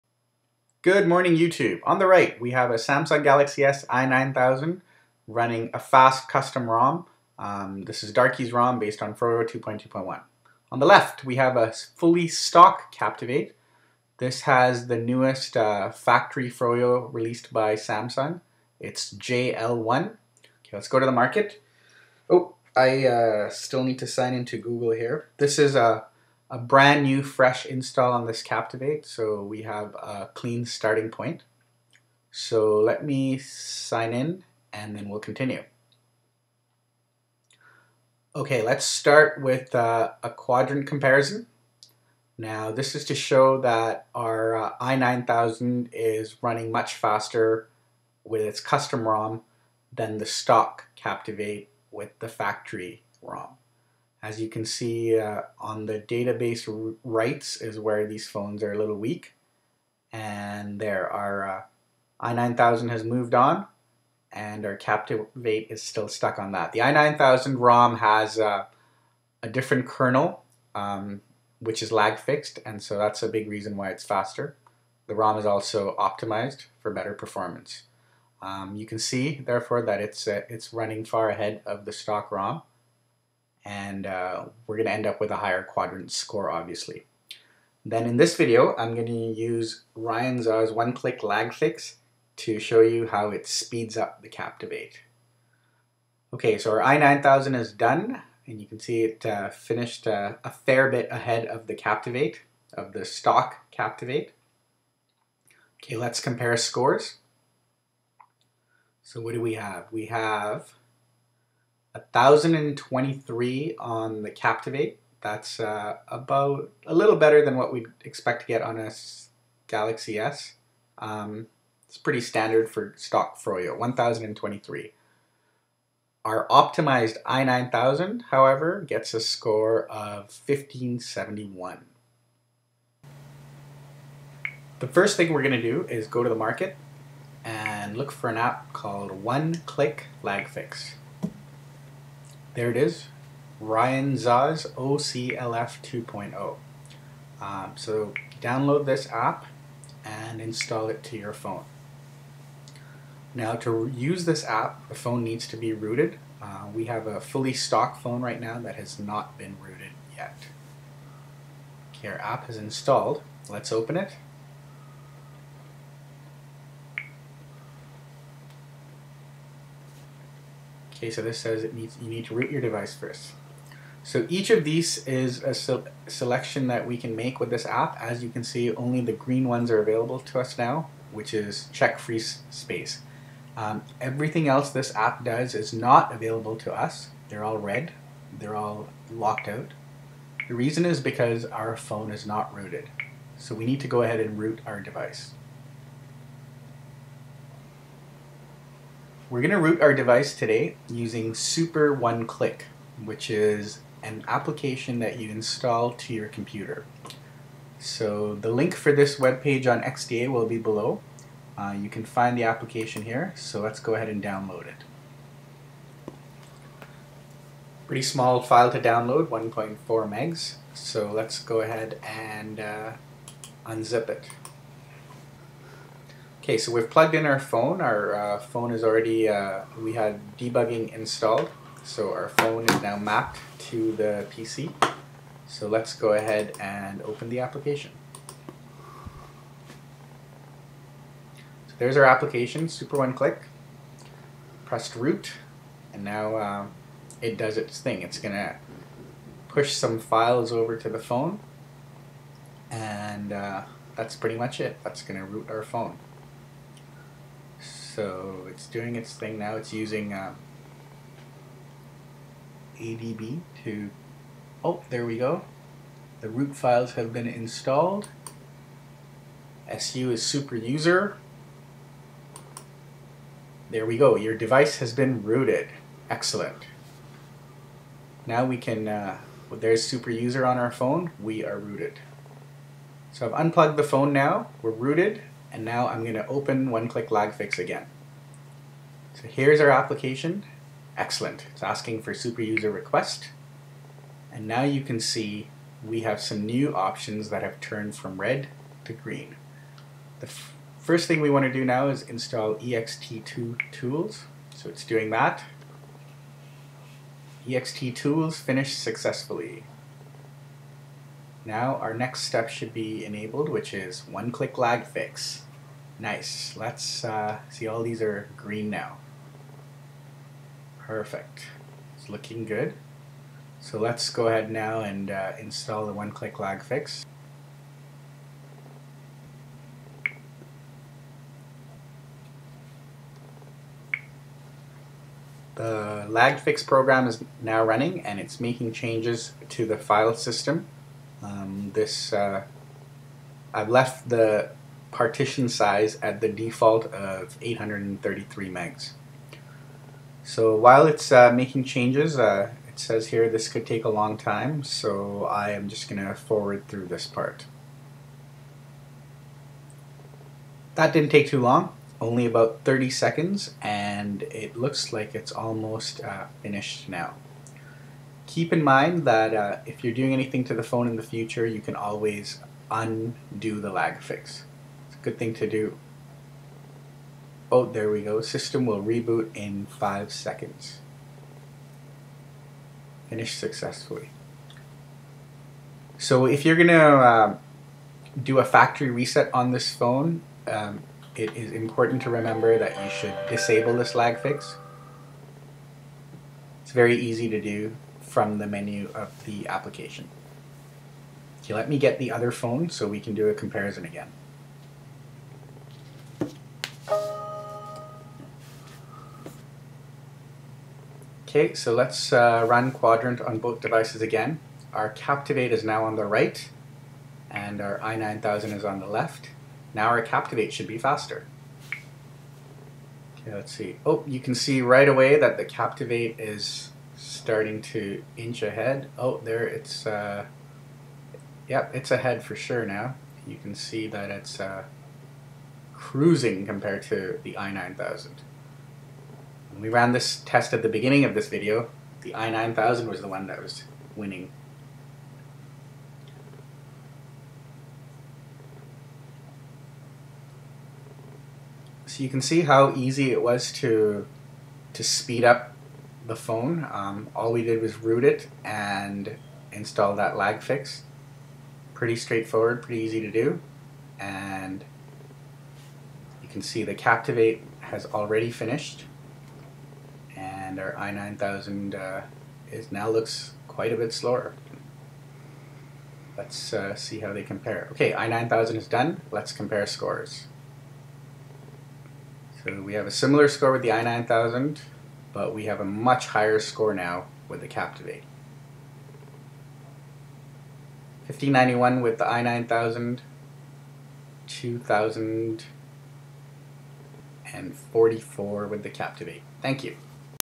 How to Root and Lag Fix Your Samsung Galaxy S Captivate
Supercharge your Samsung Galaxy S Captivate using RyanZA's One Click Lag Fix and SuperOneClick.

You will need to reboot the phone after you root with SuperOneClick